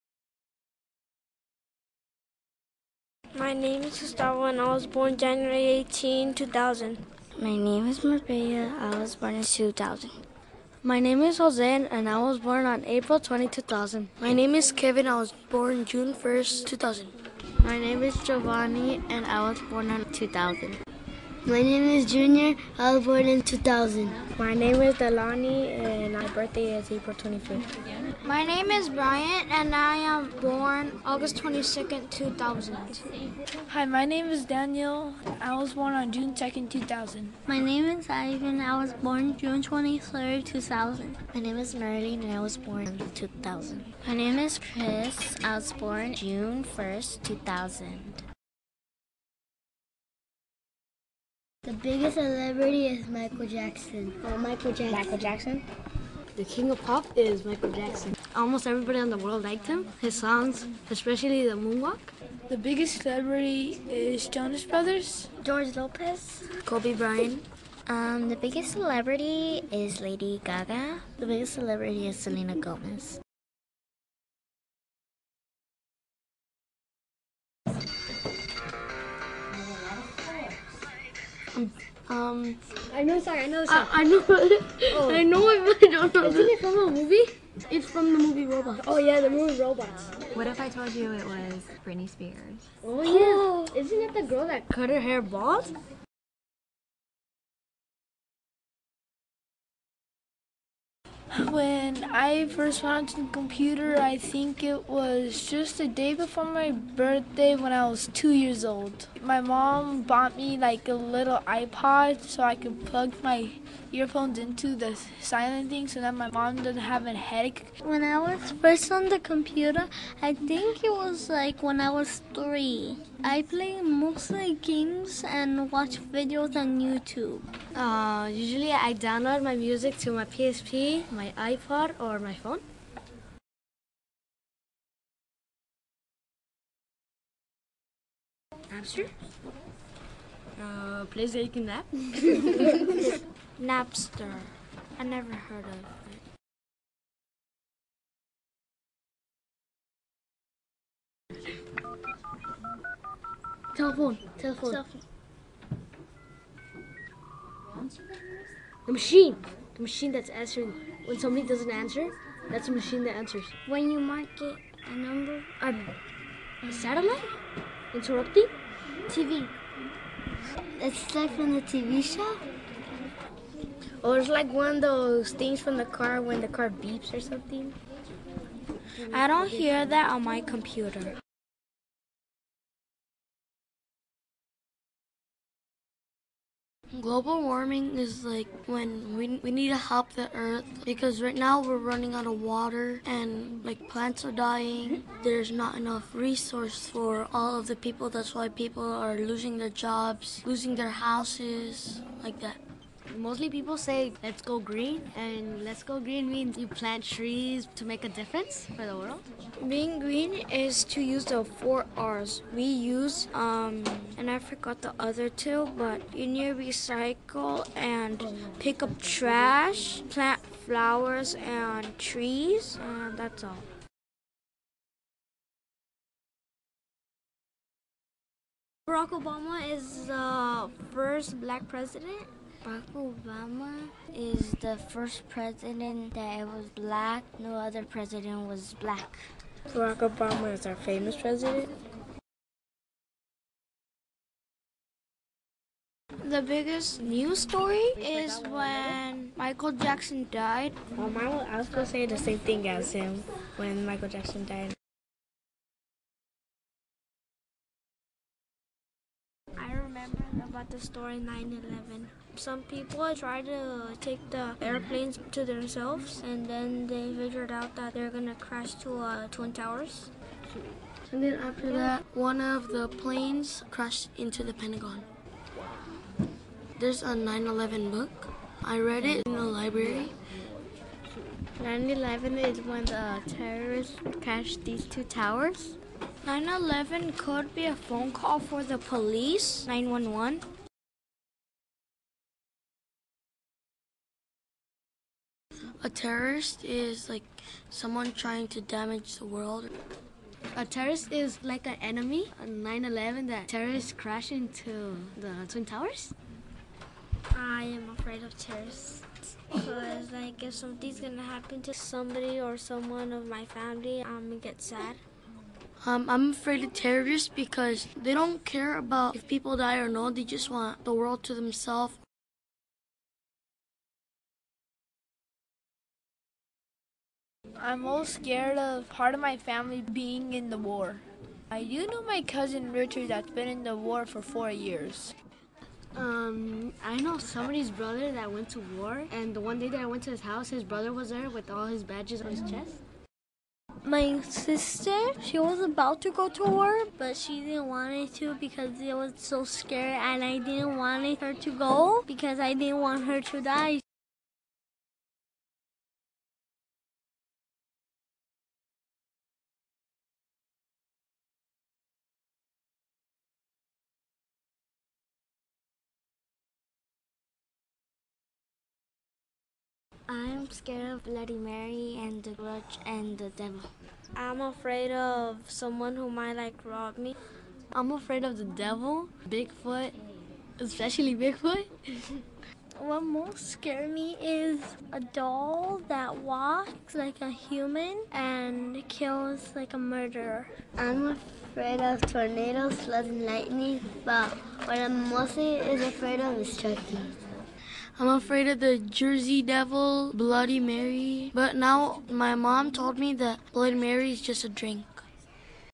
The decade according to 9-year-olds
The decade according to 9-year-olds By allison louie-garcia.
I Thought This Was Cute. I Mean No Copy Right Infridgment. If So
Please Let Me Know And Ill Remove It.